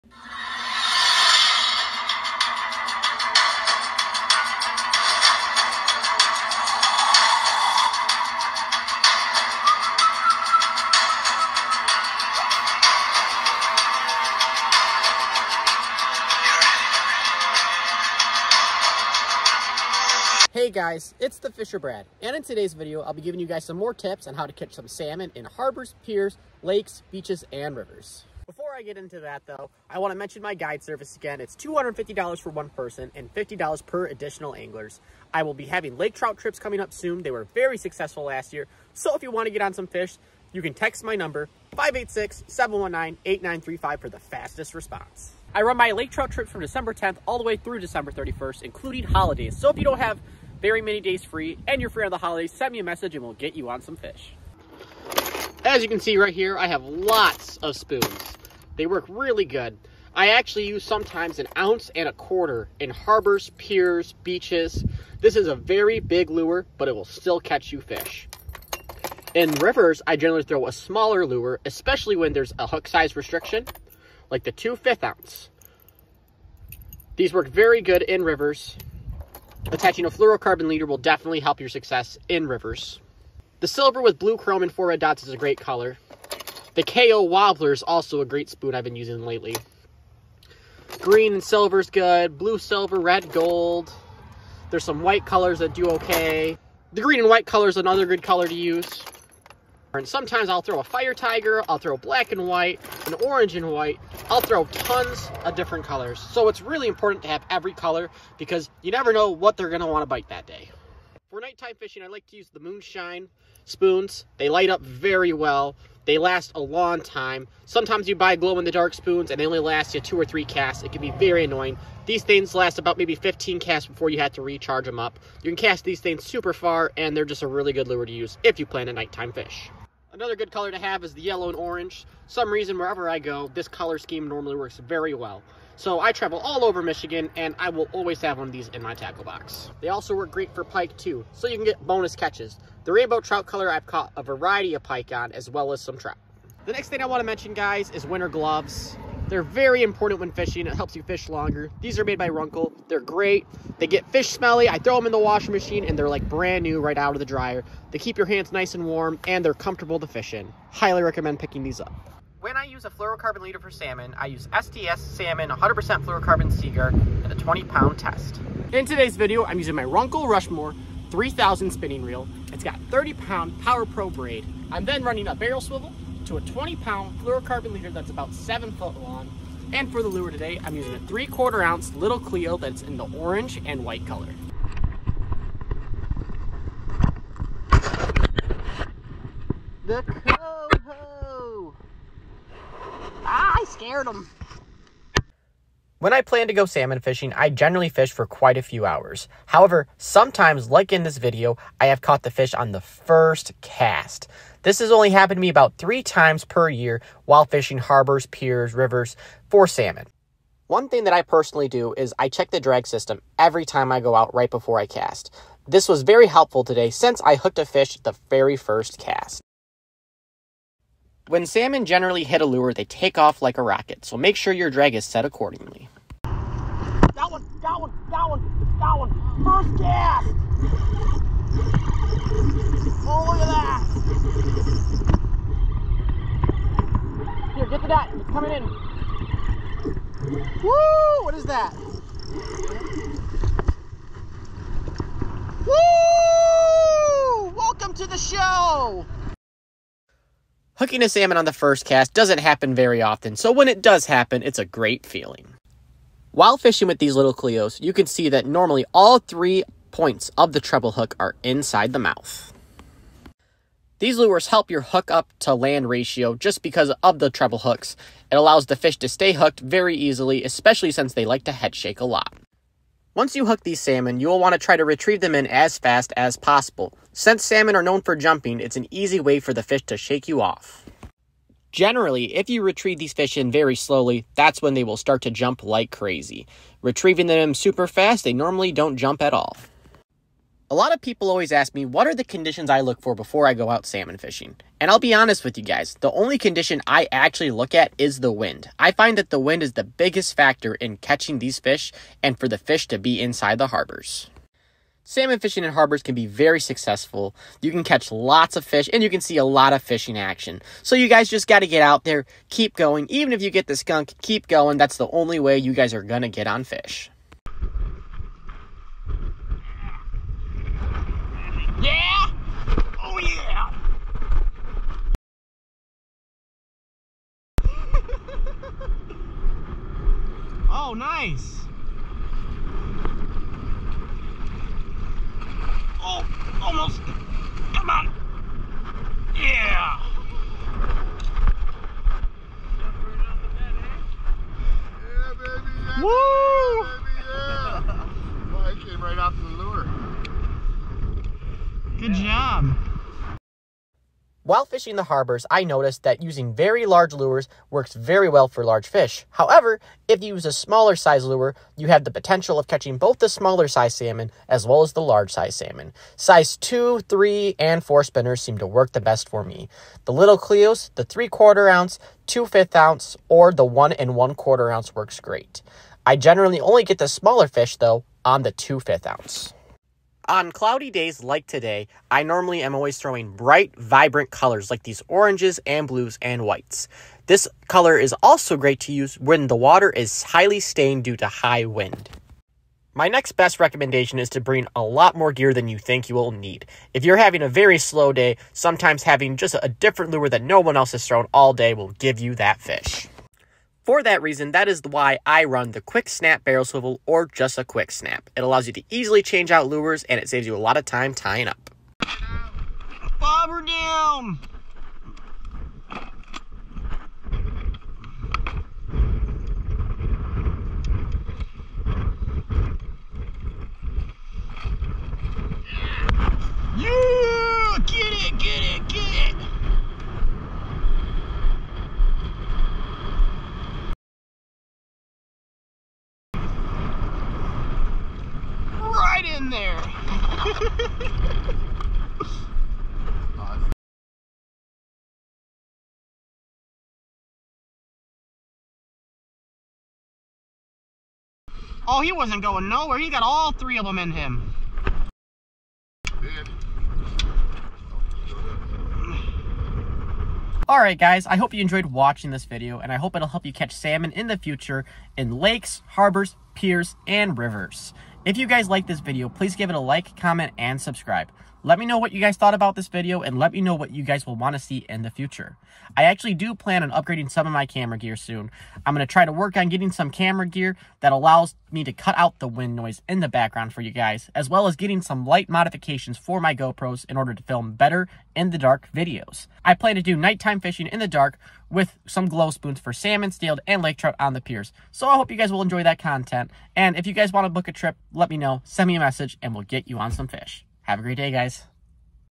Fall Harbor Salmon Fishing For Hard To Bite Fish! Little Cleo Bite! River, Pier, Beach Fishing Tips!
In this video I go over how to catch hard to bite salmon in harbors.

Runcl Rushmore 3000

Runcl JayClaw cold weather gloves

15%off code BD2RUNCL2